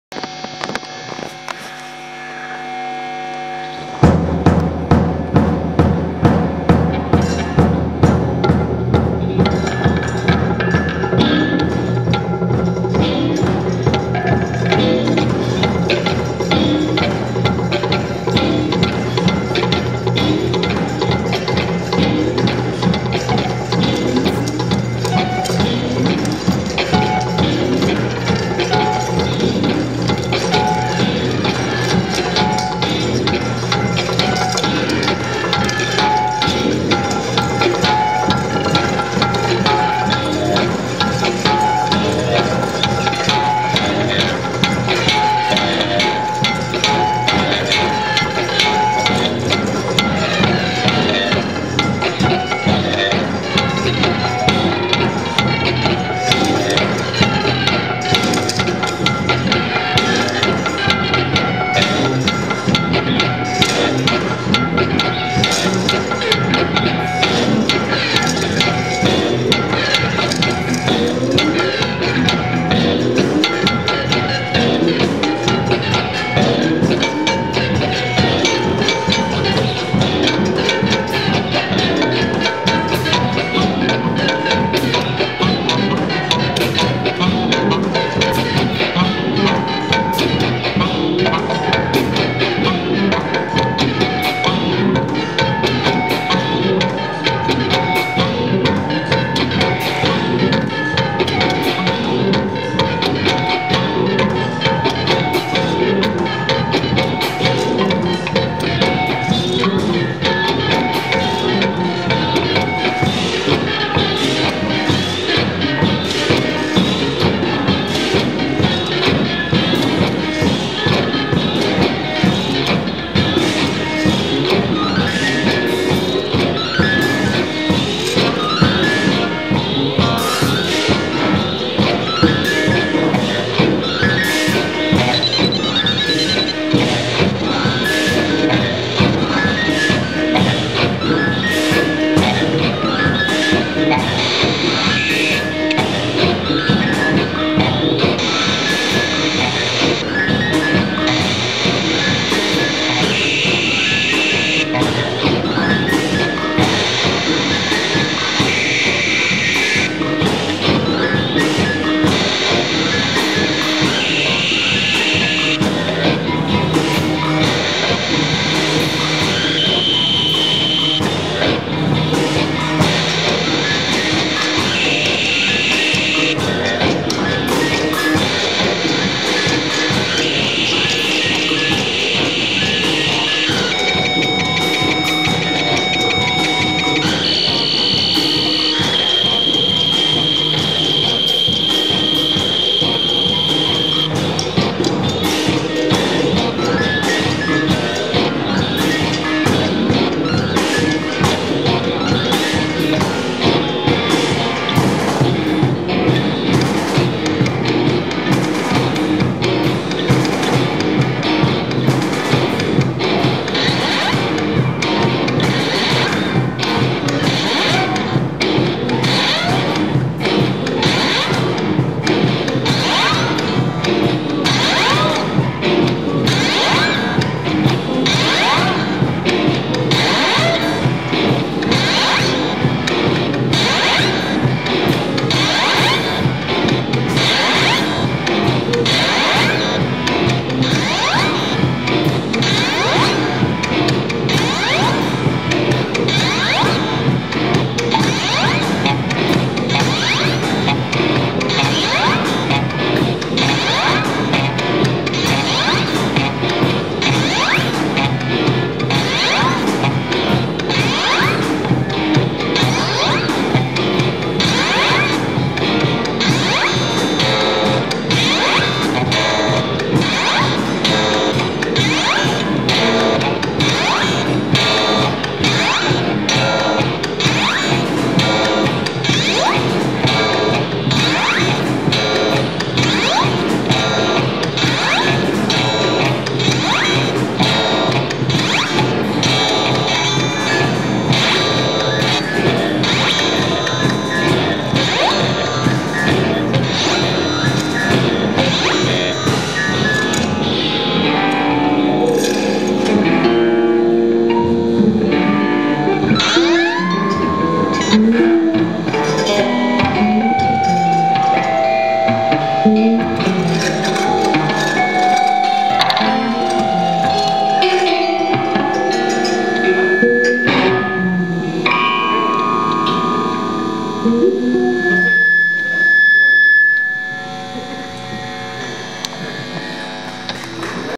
Percussion.wmv
Track using percussion instruments and homeswinger guitars in percussive mode. From Yuri Landman's homeswinger workshop - pat of NUStival